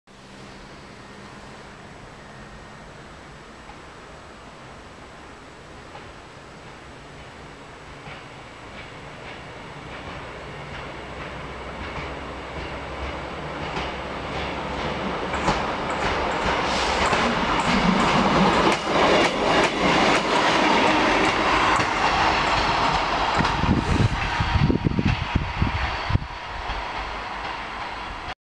prujezd 814 060 os 18415 Pacov
Průjezd regionovy 814 060 os.vl. 18415 za Pacovem
Točeno dne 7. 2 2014 foťákem Canon PowerShot A2300 HD